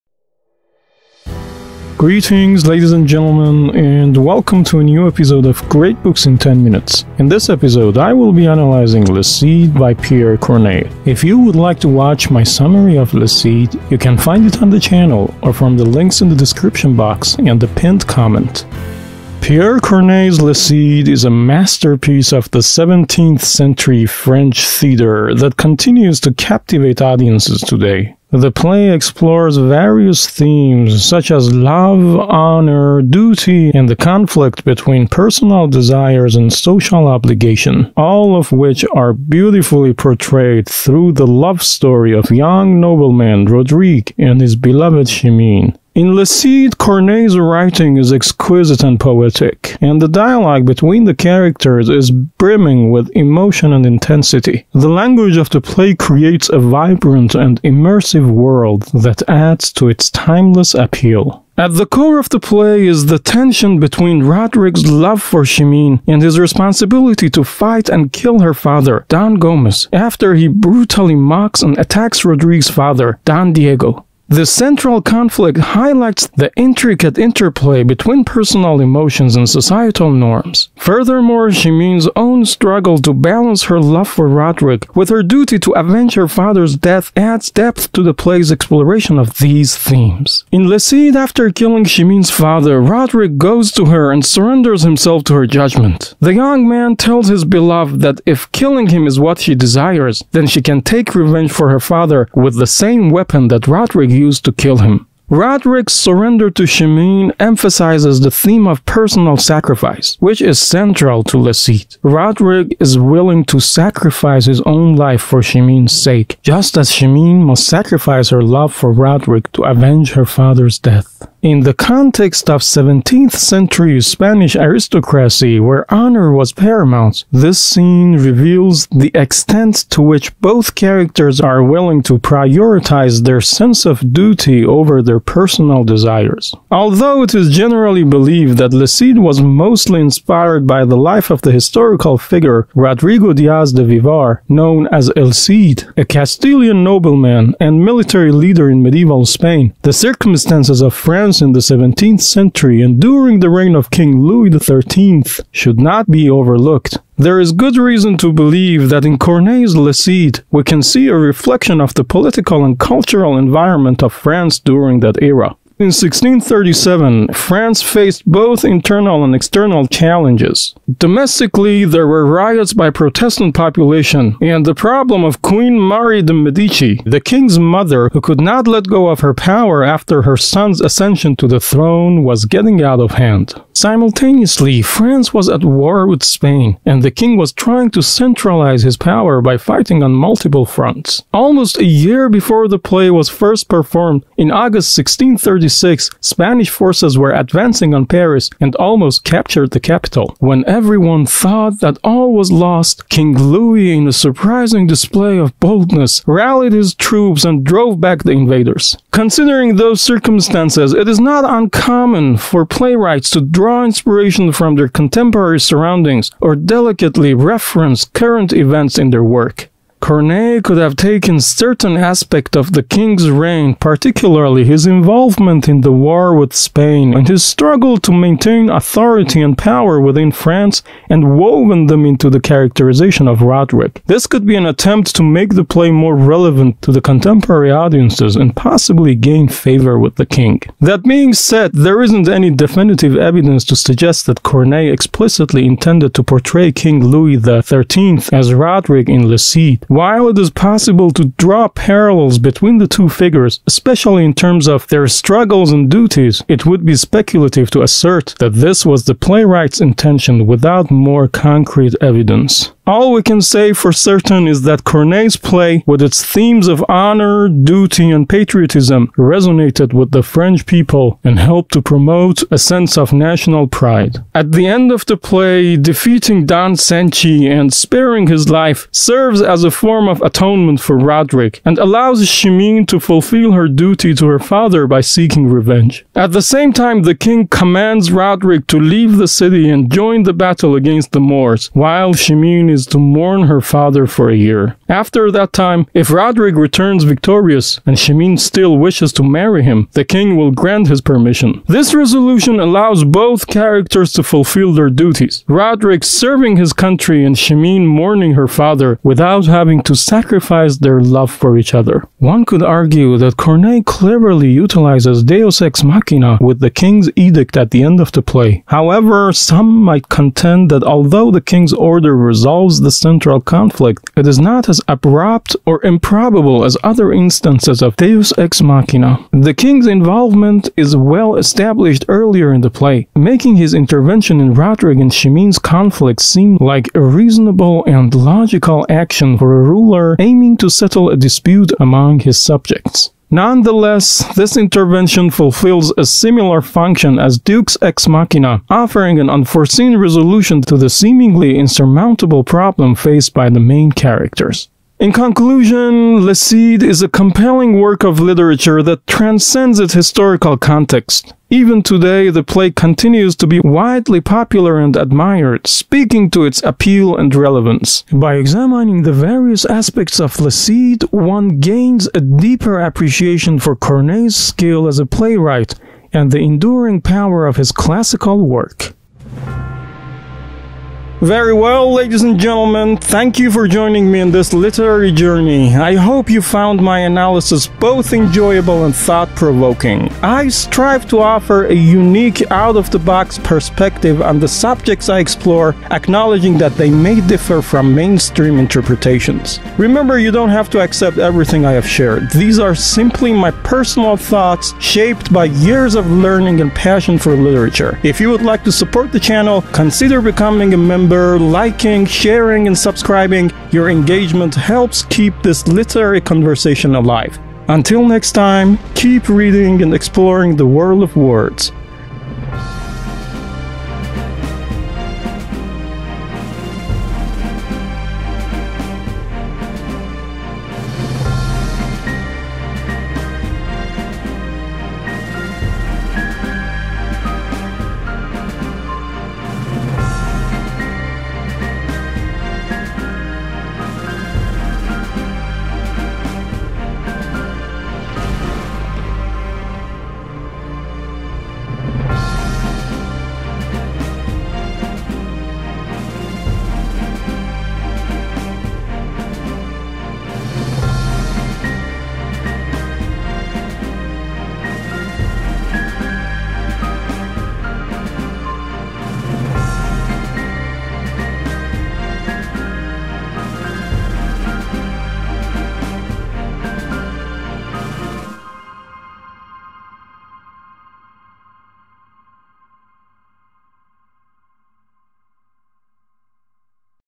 Understanding Le Cid | Exploring the Classics Series | Season One - Episode Three
🟢You can now download the free companion app for this channel and access trivia quizzes for all the episodes, free ebooks and more :
In this episode of Great Books in 10 Minutes, We will analyze Pierre Corneille's timeless masterpiece, "Le Cid." In this exploration, we delve deep into the intricacies of this renowned French Pla, dissecting its themes, characters, and the genius of Corneille's storytelling. Join me as we unravel the layers of this classic work and uncover the profound insights it offers into human nature and society.